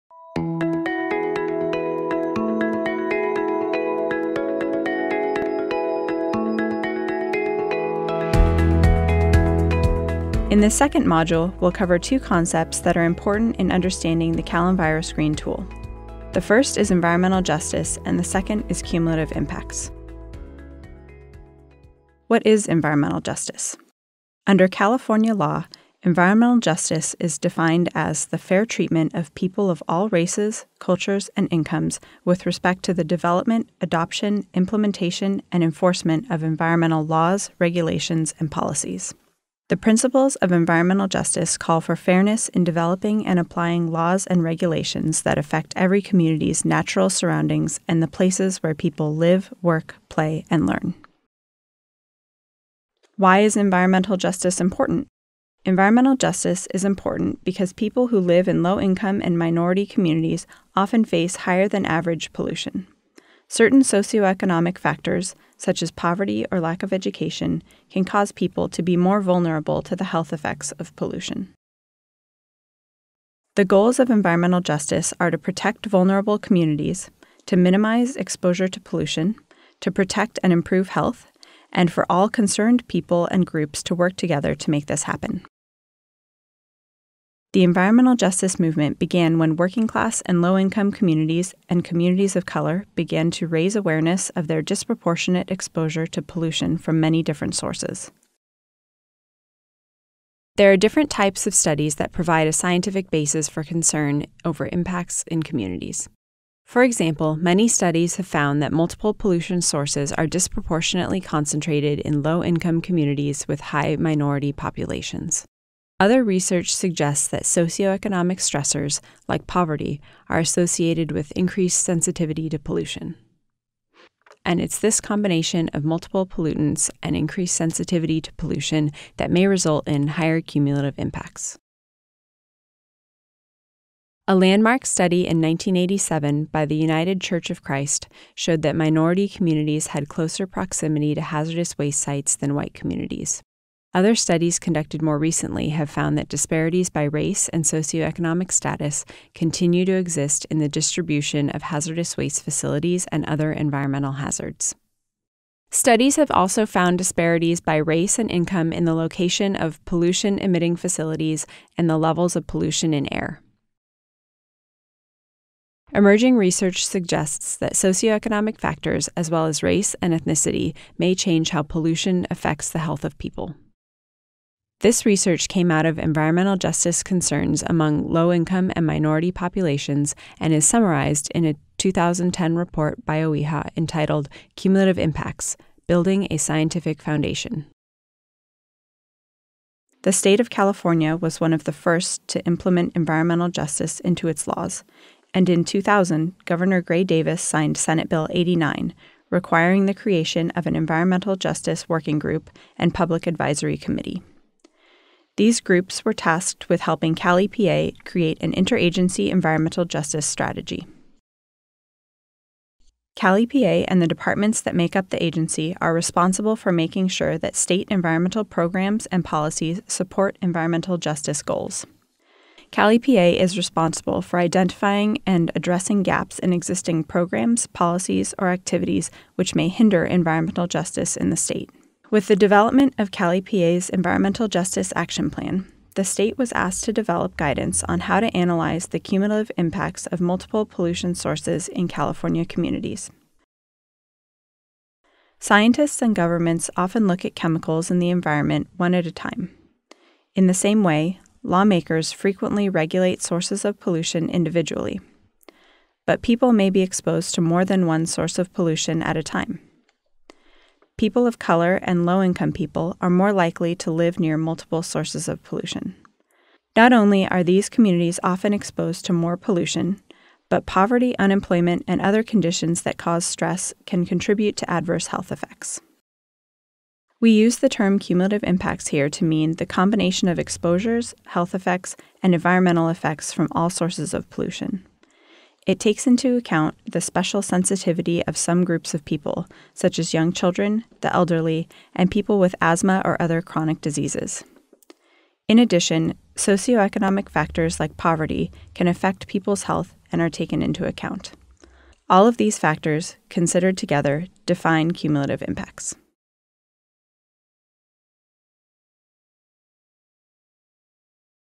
2. Environmental Justice and Cumulative Impacts
Module two gives an overview of the topics of environmental justice and cumulative impacts.